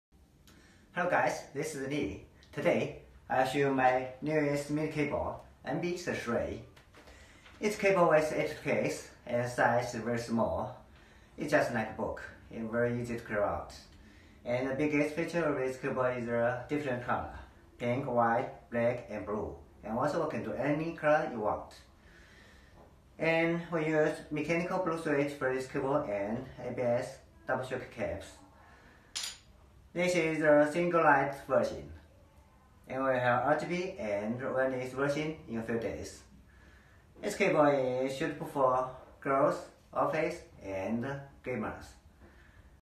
Cute Girls Pink black white blue mini 82 keys mechanical keyboard for office and gamer
This is YIQI NB newest mini 82 keys mechanical keyboard, there are many colors, like pink, white, black, blue ect. we also accept OEM brand, customize a color you like. This keyboard with blue switch, double Injection keycaps.